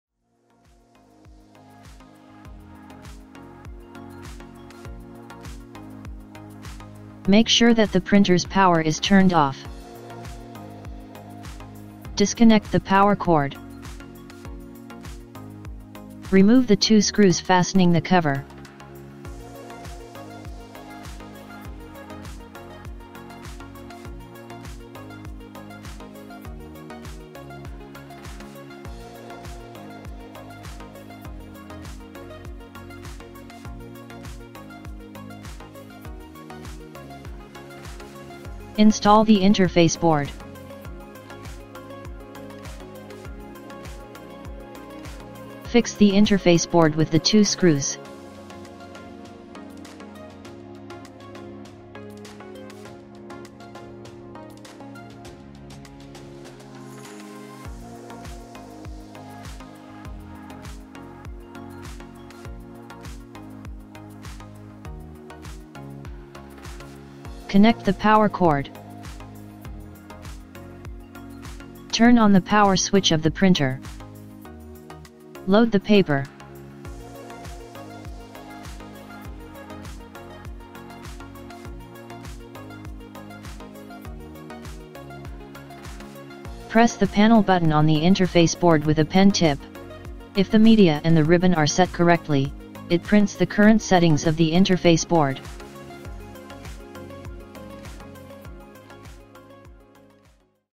CL-S400DT Interface board Installation
We will explain the procedure for installing the interface board for CL-S400DT.